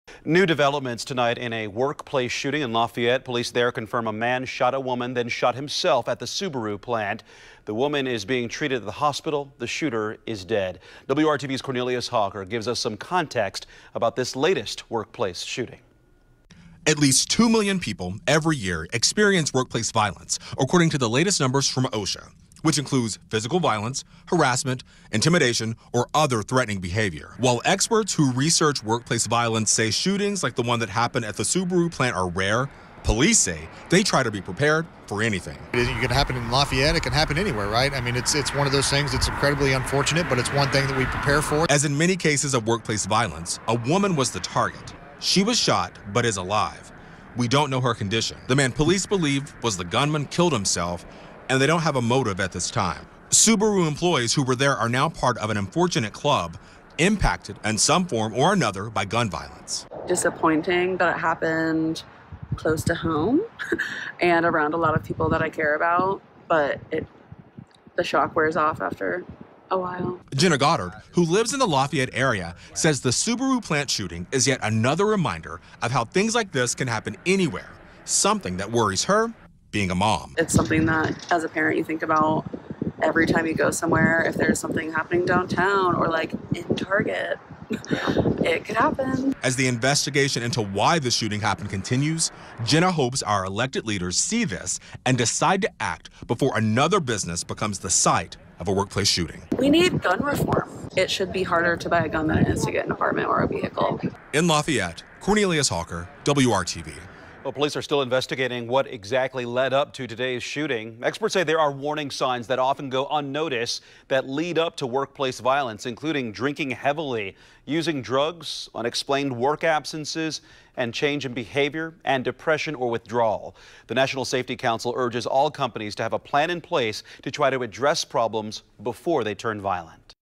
Woman injured in shooting at Subaru plant in Lafayette
A woman was injured after she was shot by a man Monday at the Subaru facility in Lafayette, according to police.